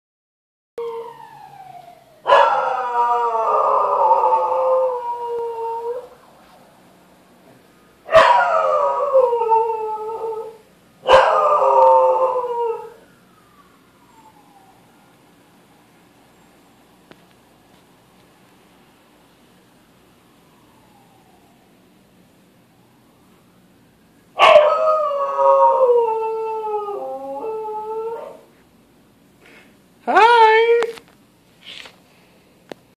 Rocky howls at a siren
Took months but I finally managed to get a good recording!